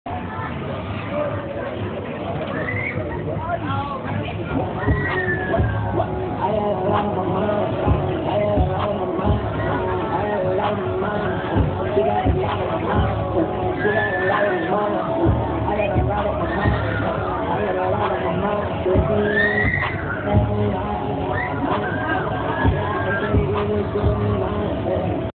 The most terrifying thing I ever rode
my sister filmed this about 2 years ago at the tampa state fair also I am on that ride lol it was fun but terrifying especially getting hung upside down for so long